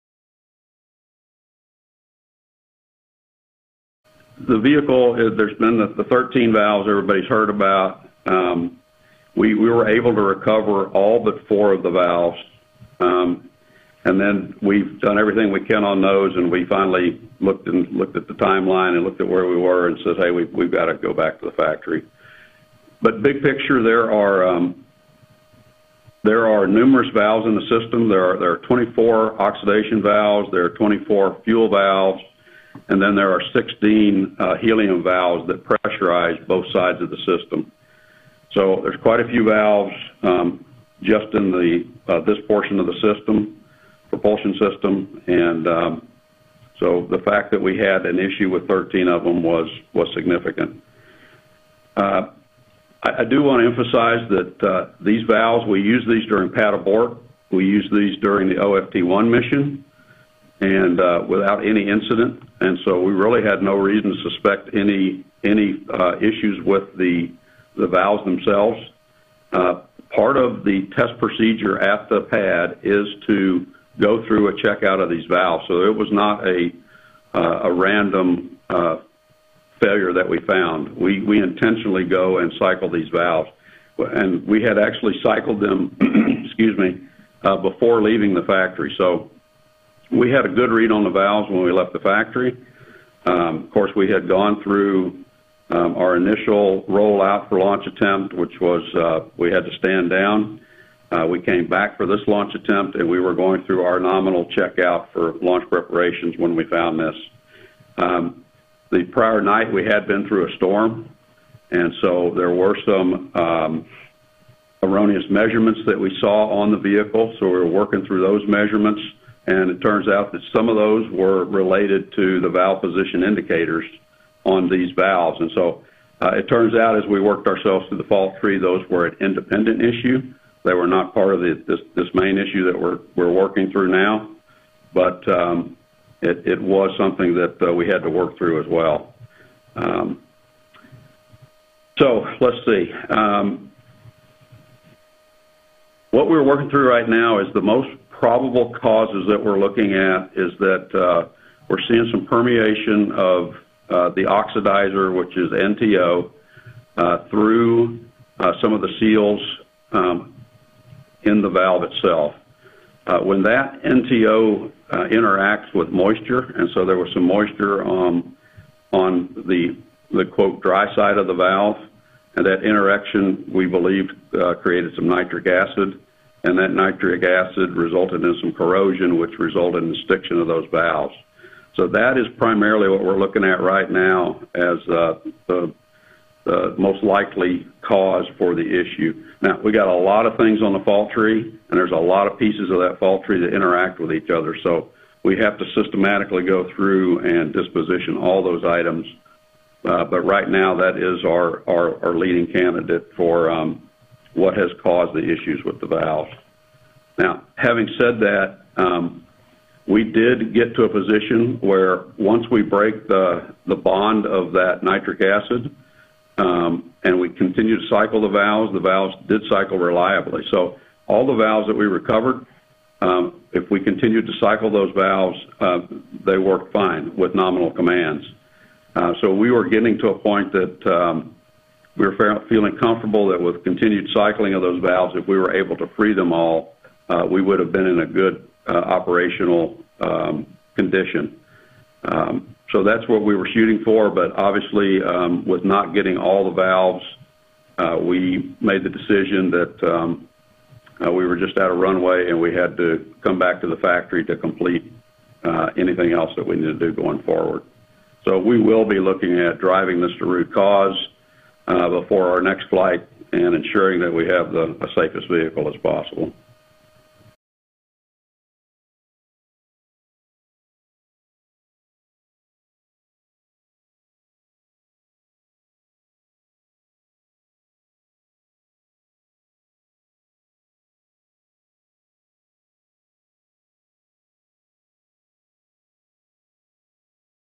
Status of the Starliner OFT-2 spacecraft
The issues experienced by Boeing’s Crew Space Transportation (CST)-100 Starliner spacecraft for the Orbital Flight Test-2 (OFT-2) mission presented by John Vollmer, Vice President and Program Manager of Boeing’s Commercial Crew Program.
Credits: NASA/United Launch Alliance/Boeing